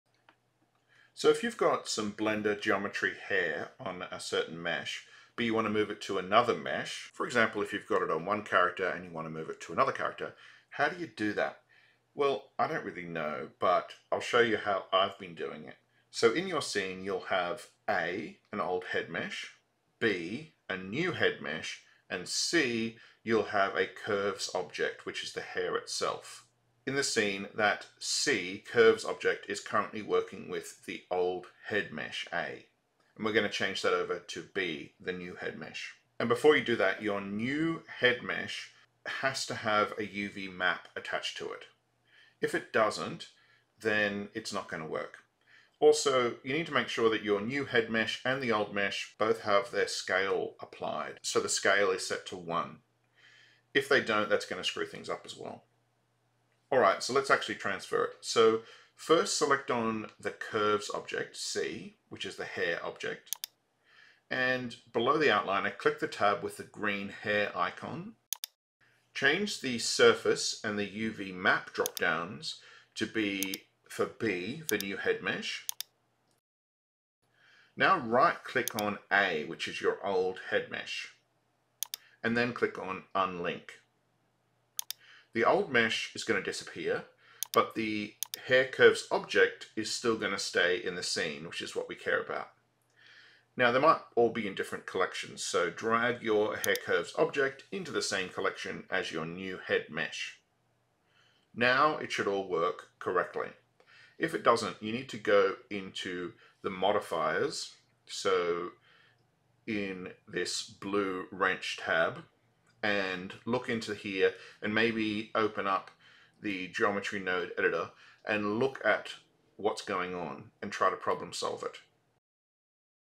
How To Move Blender Geometry Hair To New Character Or Mesh
I've struggled a bit with this, so I wanted to make a quick video showing how to move "Curves" hair objects (blender geometry hair) to a different mesh or character.

I don't know much about Geometry Nodes, but I couldn't find this answer clearly anywhere. This seems to work for me. :-)